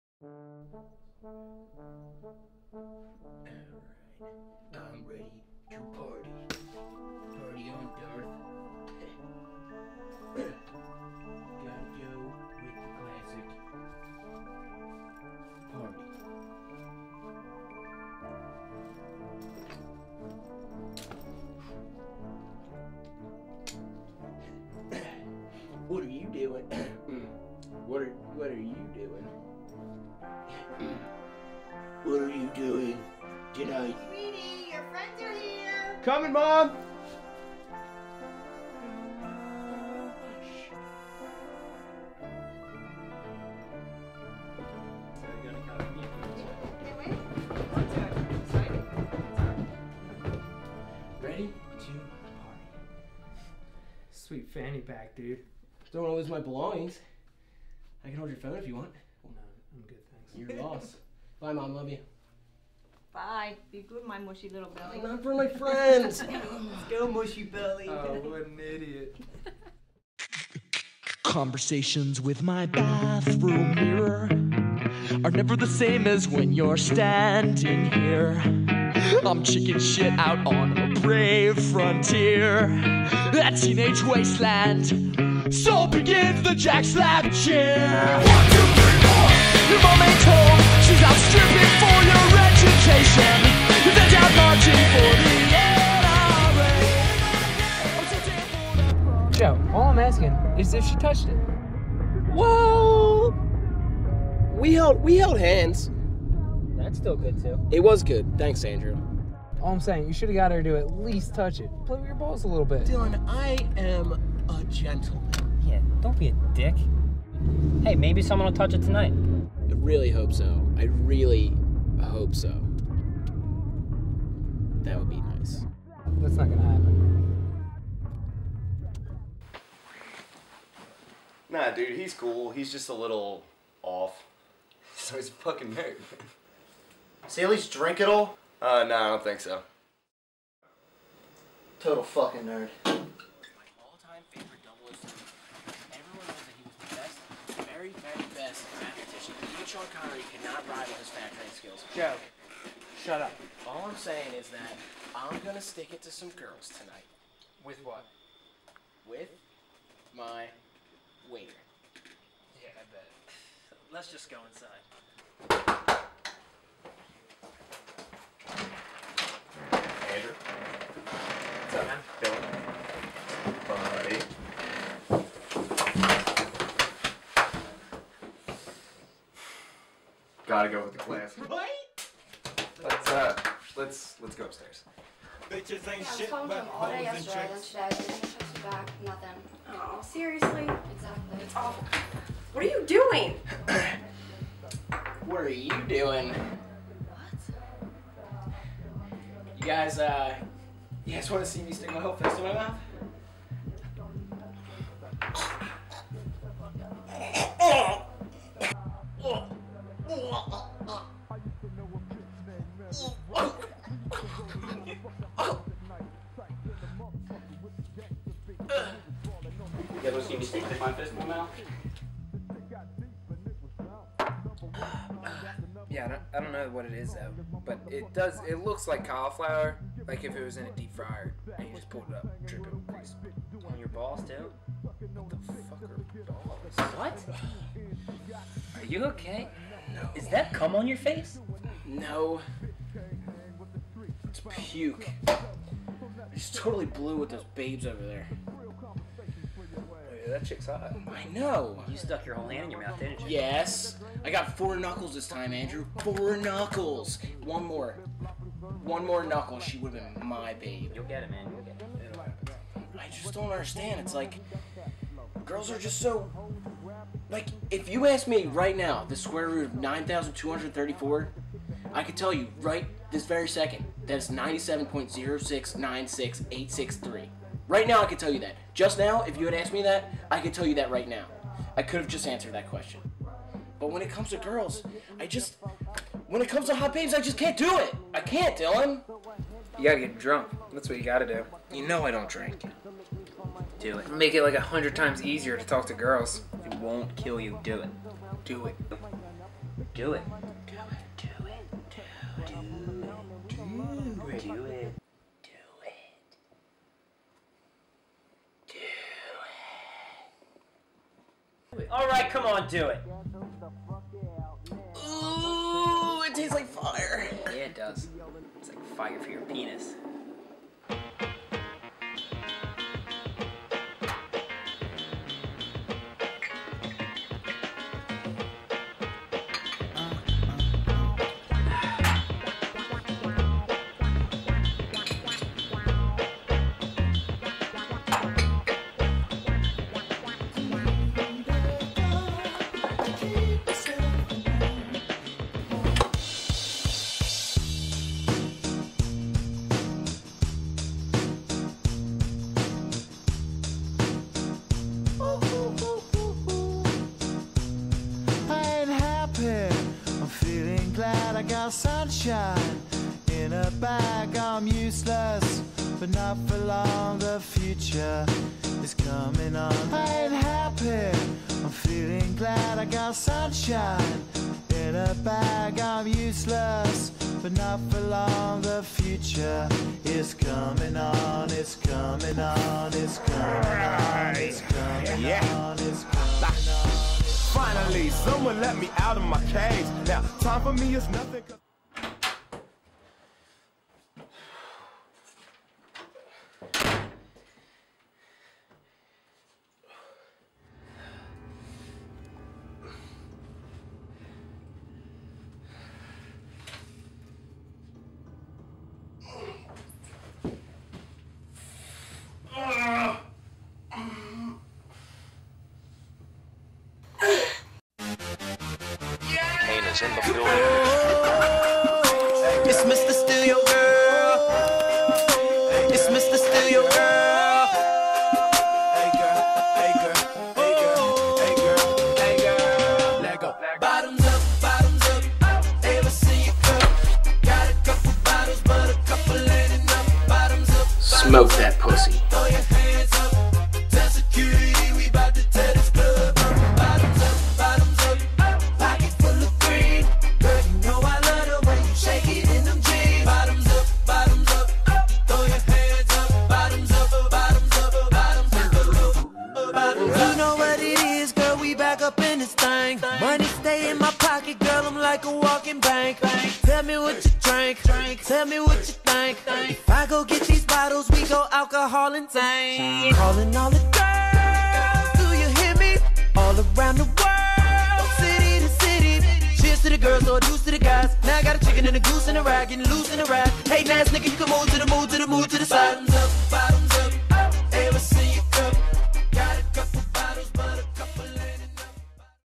The Origins of Mr. Steal Yo Girl
The first production from BAD FILMS

A coming of age story about a true baller to be.

Written by Tim Murray and Dylan Moore
Directed by The Bad Films dudes
Camera: Tim Murray
Edited by Joe Cartlidge and Dylan Moore

Based on the character created by Sam Coyle